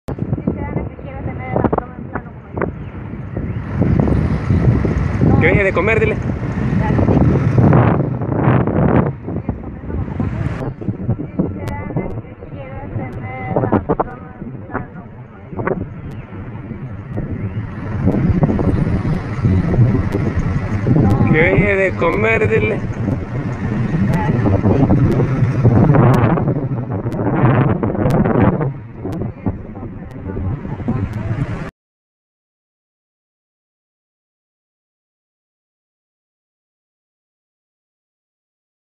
Ufo Ovni 2 Esferas 06 07 2015 HD 1080i
Ufo Ovni 2 Esferas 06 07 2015 HD 1080i Ver video Full pantalla. 1 pasa  serca del poste y la 2 se queda parada alado de la palma de coco.